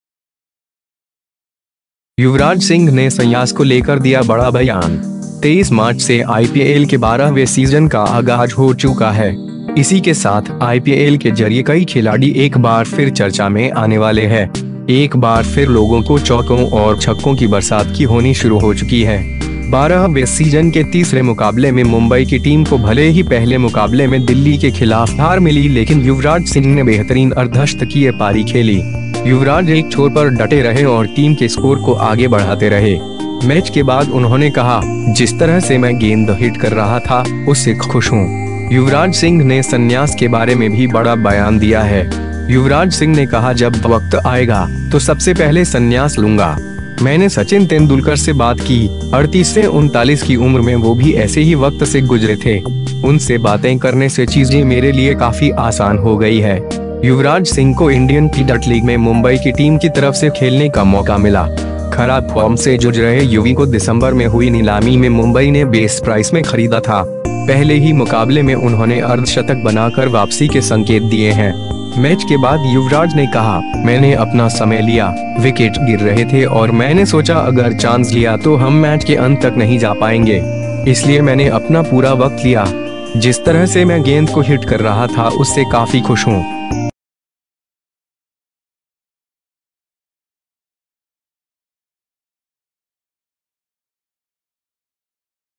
युवराज सिंह ने सन्यास को लेकर दिया बड़ा बयान
23 मार्च से आईपीएल के 12 वे सीजन का आगाज हो चूका है। इसी के साथ आईपीएल के जरिये कई खिलाडी एक बार फिर चर्चा में आने वाले है। एक बार फिर लोगों को चौकों और छक्कों की बरसात की होनी शुरू हो चुकी है।

12 वे सीजन के तीसरे मुकाबले में मुंबई की टीम को भले ही पहले मुकाबले में दिल्ली के खिलाफ हार मिली लेकिन युवराज सिंह ने बेहतरीन अर्धशतकीय पारी खेली। युवराज एक छोर पर डटे रहे और टीम के स्कोर को आगे बढ़ाते रहे। मैच के बाद उन्होंने कहा, जिस तरह से मैं गेंद हिट कर रहा था, उससे खुश हूं।

युवराज सिंह ने संन्यास के बारे में भी बड़ा बयान दिया है। युवराज सिंह ने कहा ”जब वक्त आएगा, तो सबसे पहले संन्यास लूंगा। मैंने सचिन तेंदुलकर से बात की, 38-39 की उम्र में वो भी ऐसे ही वक्त से गुजरे थे। उनसे बातें करने से चीजें मेरे लिए काफी आसान हो गई है।”

युवराज सिंह को इंडियन टी20 लीग में मुंबई की टीम की तरफ से खेलने का मौका मिला। खराब फॉर्म से जुझ रहे युवी को दिसंबर में हुई नीलामी में मुंबई ने बेस प्राइस में खरीदा था। पहले ही मुकाबले में उन्होंने अर्धशतक बनाकर वापसी के संकेत दिए हैं।

मैच के बाद युवराज ने कहा, ”मैंने अपना समय लिया, विकेट गिर रहे थे और मैंने सोचा अगर चांस लिया तो हम मैच के अंत तक नहीं जा पाएंगे। इसलिए मैंने अपना पूरा वक्त लिया। जिस तरह से मैं गेंद को हिट कर रहा था उससे काफी खुश हूं।”